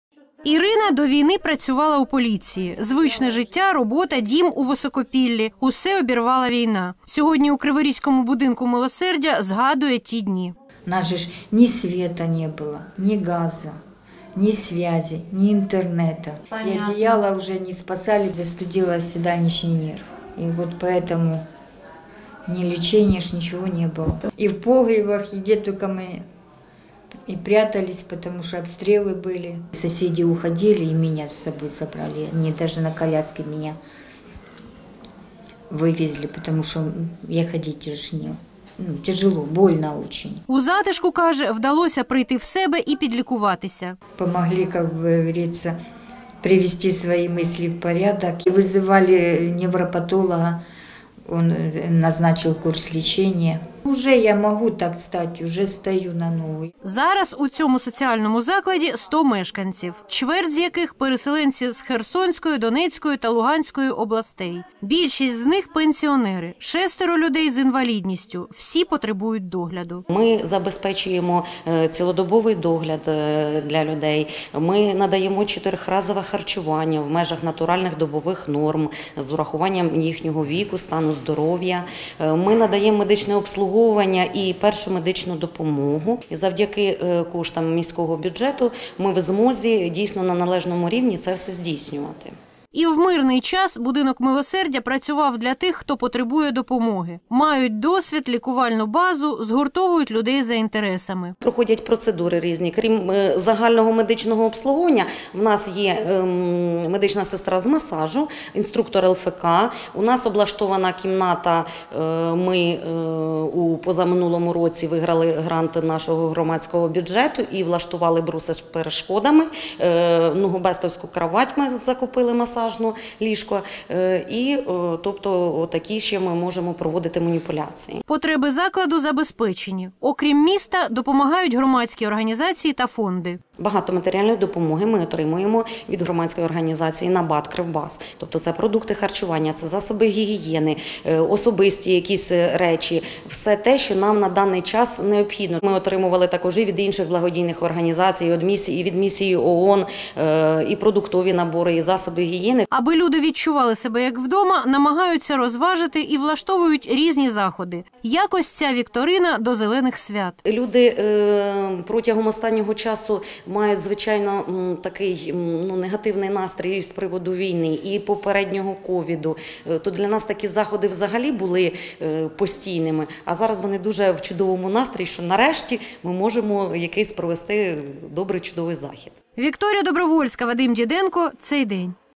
Знайшли допомогу і затишок: життя переселенців у соціальному закладі
Із перших днів повномасштабної війни прихисток у криворізькому будинку милосердя «Затишок» знаходять переселенці з окупованих ворогом територій та зони бойових дій. Мешканці цього закладу - пенсіонери та люди з інвалідністю. Про роботу колективу «Затишку» та життя їхніх підопічних розкаже Вікторія Добровольська.